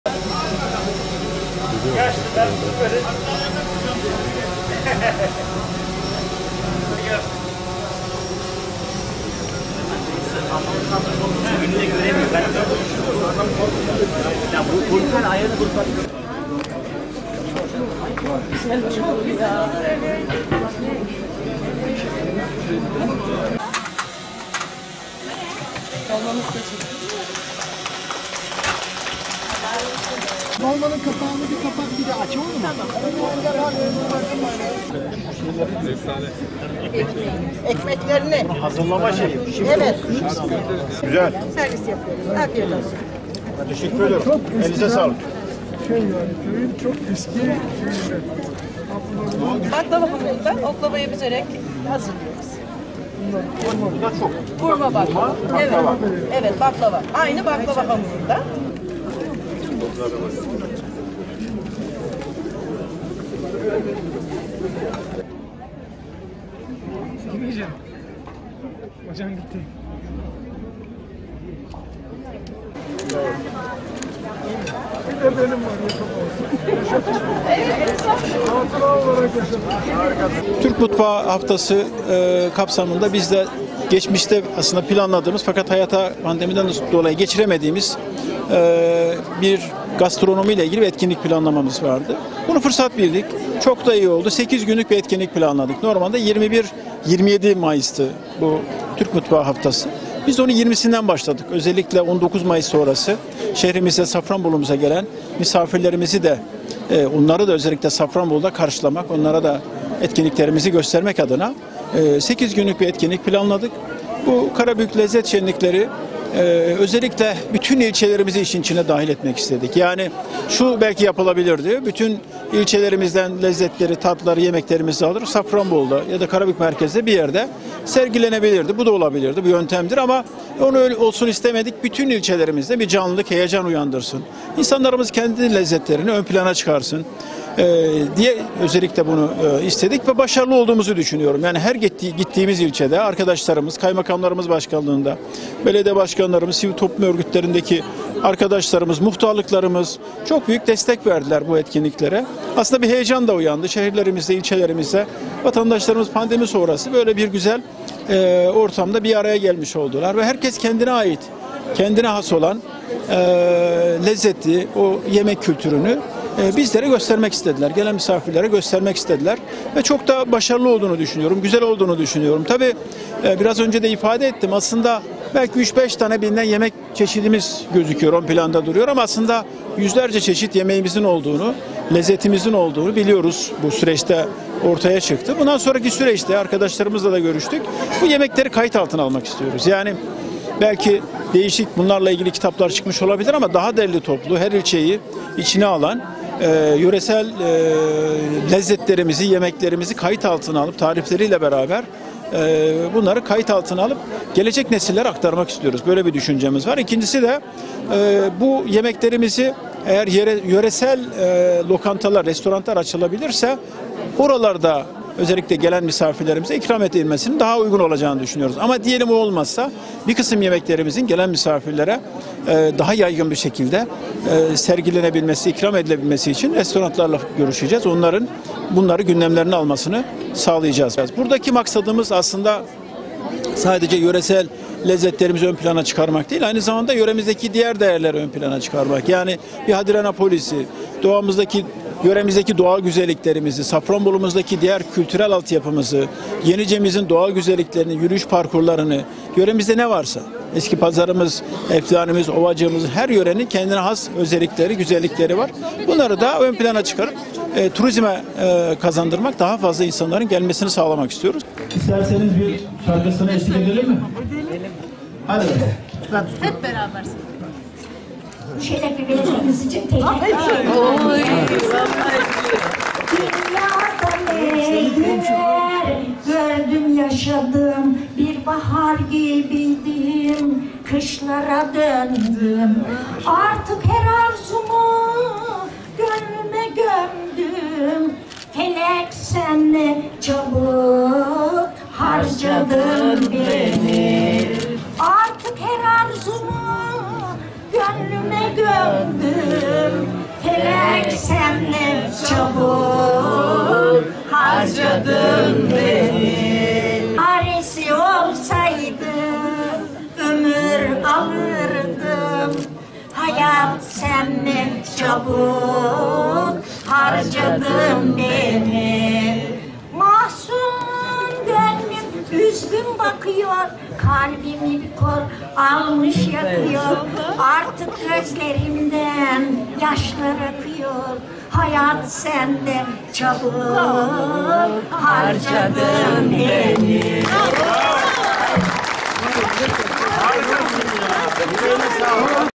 Yerel lezzetlerin tanıtıldığı ‘Lezzet Şenlikleri’ sona erdi
Karabük ve ilçelerindeki yerel lezzetlerin tanıtıldığı 8 günlük "Karabük Lezzet Şenlikleri" sona erdi.